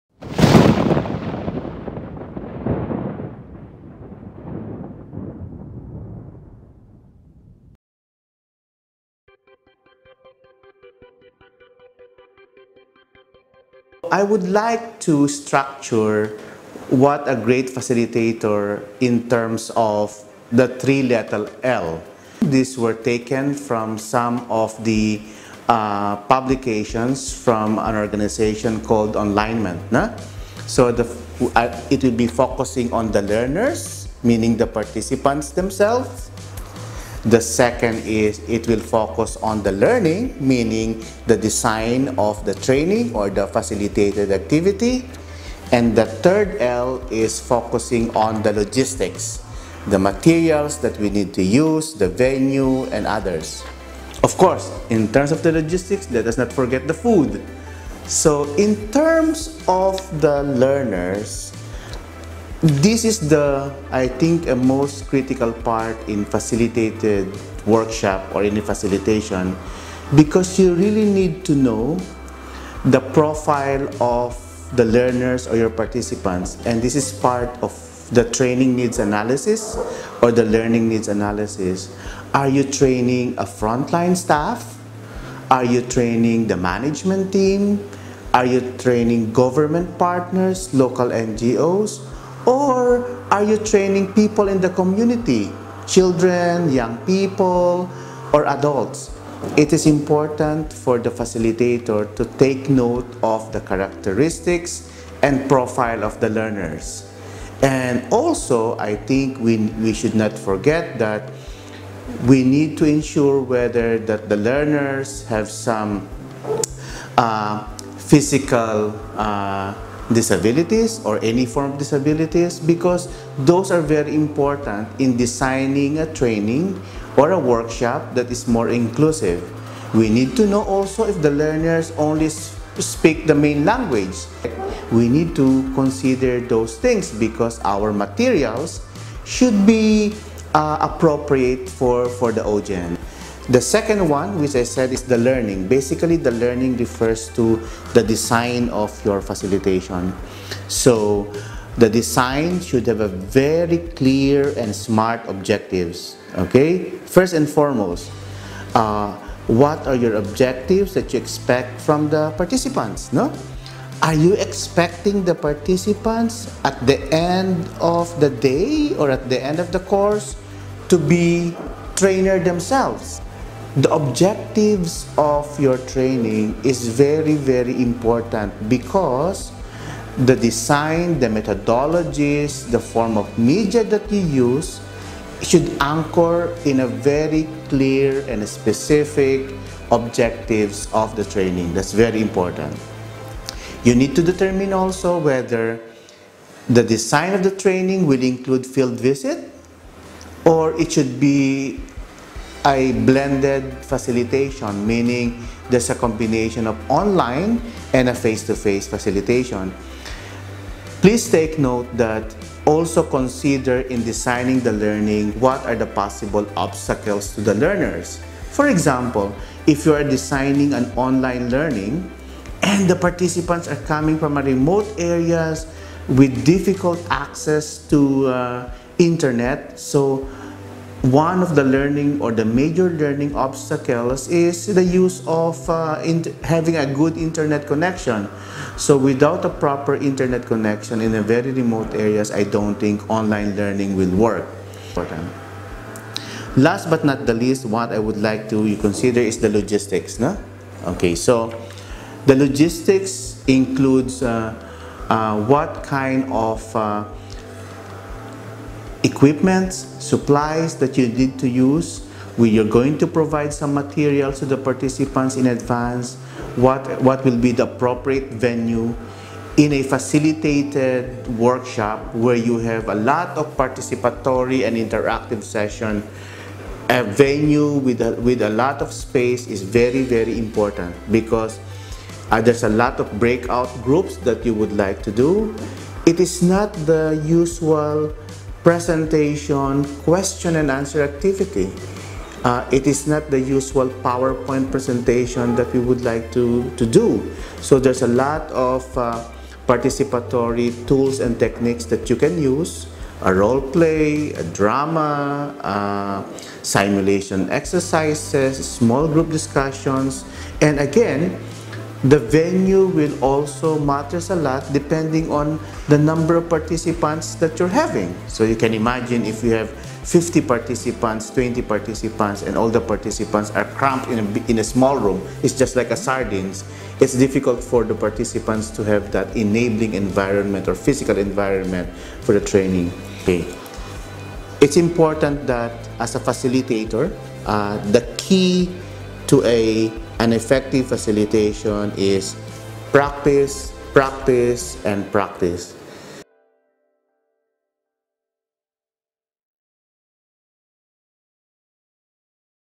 Tips Three Ls
Tips shared  by Dr. Pedrito Sandy M. Fortuna, Regional Child Centered Community Development (CCCD) Specialist - Plan International.